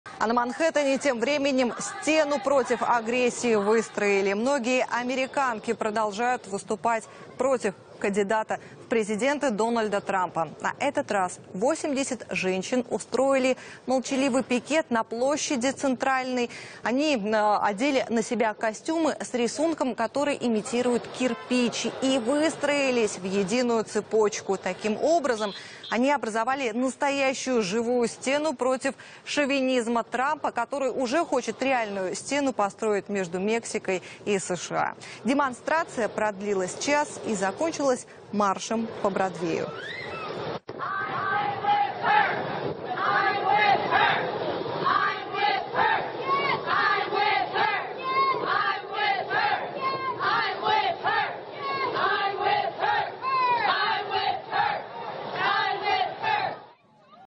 Женщины бойкотируют против Трампа
Многие американки продолжают выступать против Трампа